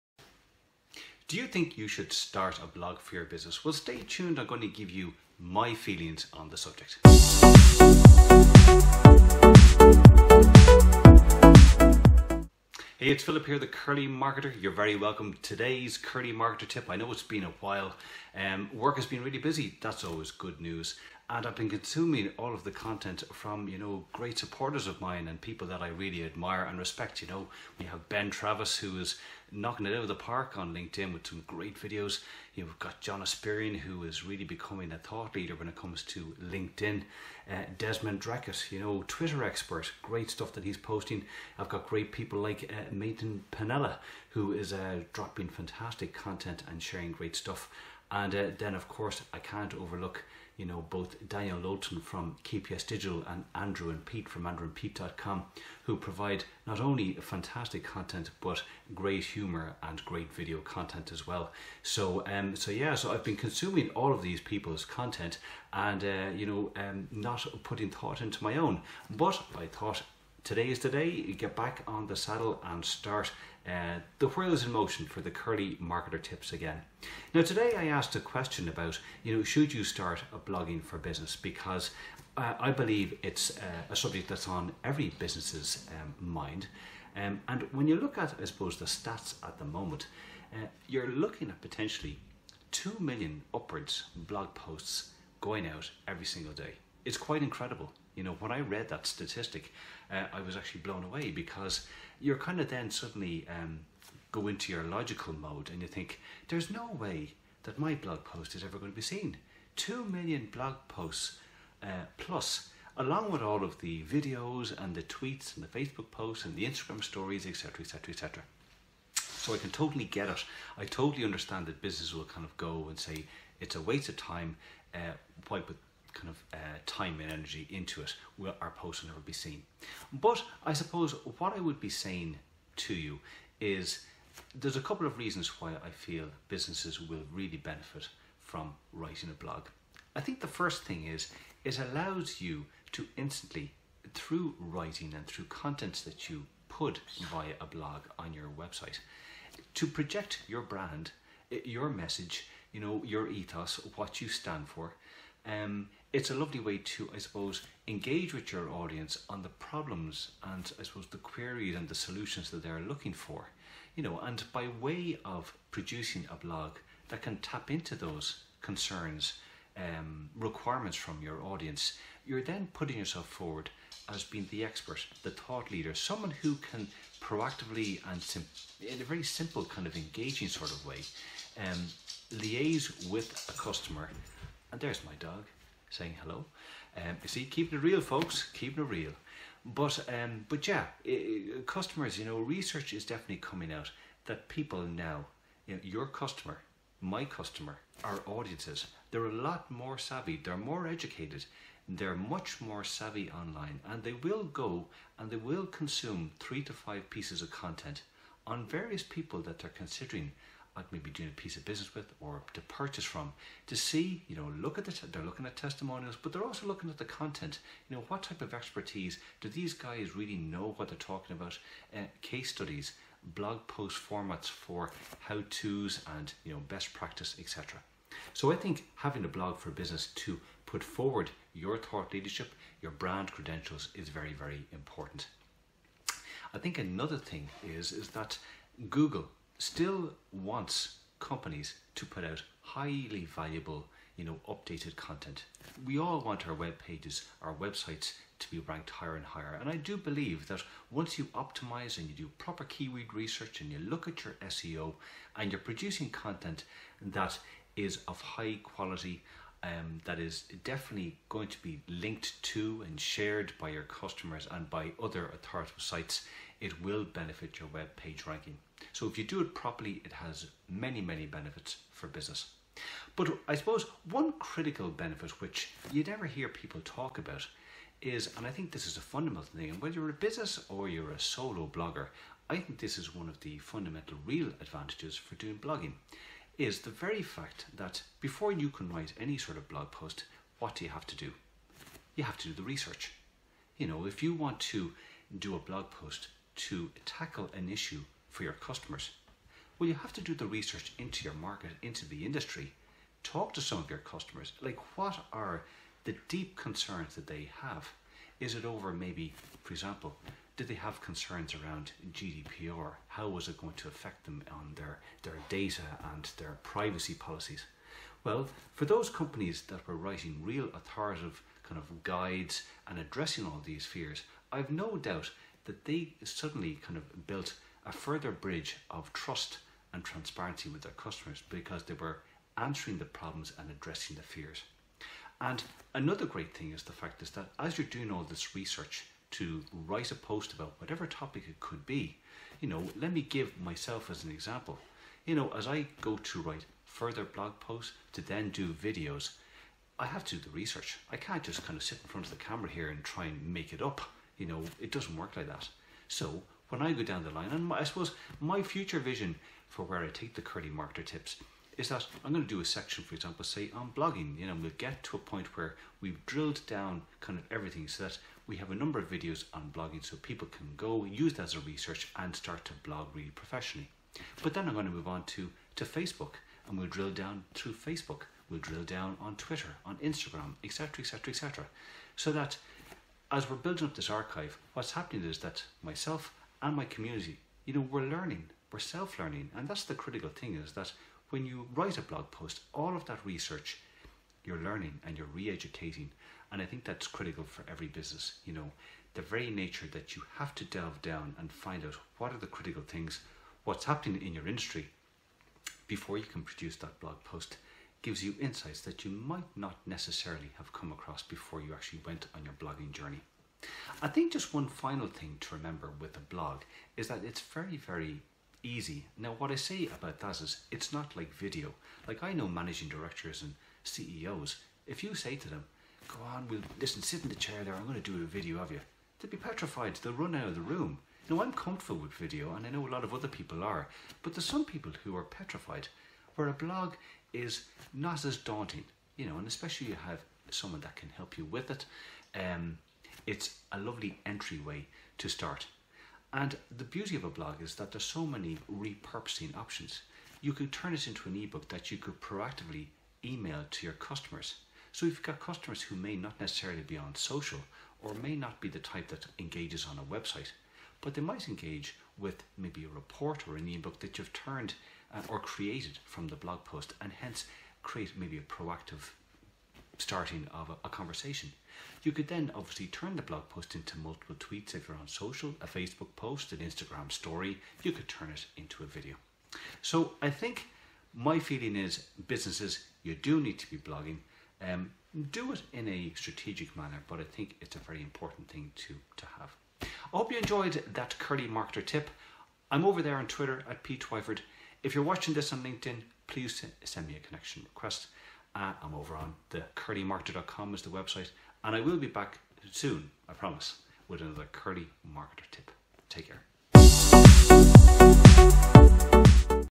Should you blog for your business success?
With over 2 million blog posts posted each day, I totally get that you’re probably thinking that if you start blogging for your business it will be a complete waste of time.

Here is a video where I give you my reasons why you should start a blog.

Looking for a guide to create a blogging strategy?  Visit this link and download my free guide